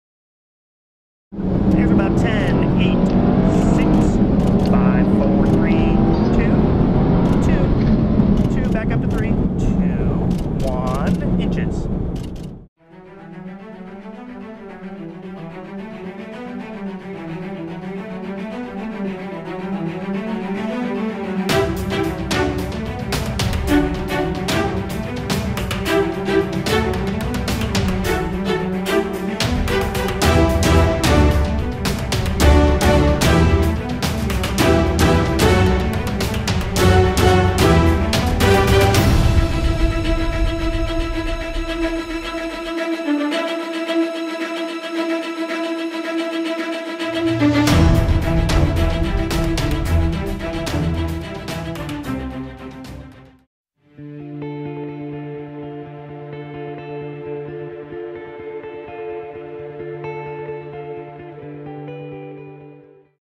Dramatic U-2 Landing Chase
Dramatic chase car footage captures the final seconds of a U-2 reconnaissance jet touchdown after a mission in 2012. Newly edited scenes catch the remarkable airframe flexing in the U-2. Come along for the ride!